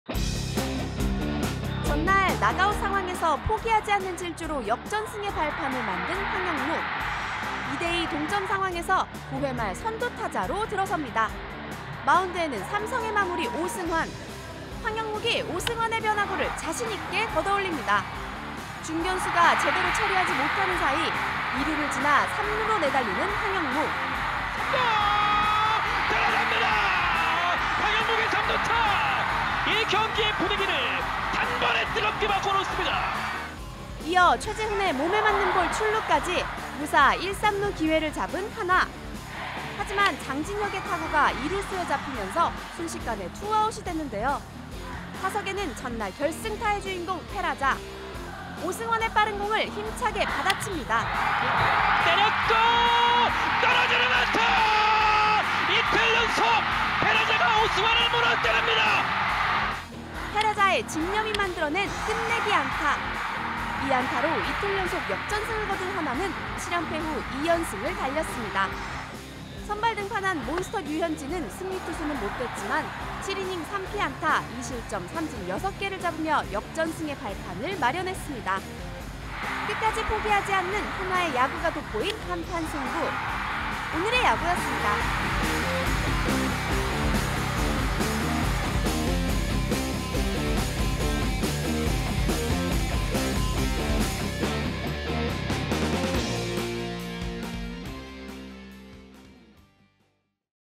황영묵은 멈추지 않았고, 페라자는 끝냈다! 한화, 이틀 연속 뒤집기 쇼｜지금 이 뉴스
한화가 끝내기 승리를 거두며 이틀 연속 삼성에 역전승을 거뒀습니다. 한화는 9회 황영묵이 3루타를 치며 기회를 잡았고, 페라자가 삼성 마무리 오승환을 상대로 끝내기 적시타를 쳐 3대 2 짜릿한 승리를 거뒀습니다. 한화는 이틀 연속 끝까지 포기하지 않고 경기를 뒤집는 저력을 보여줬습니다.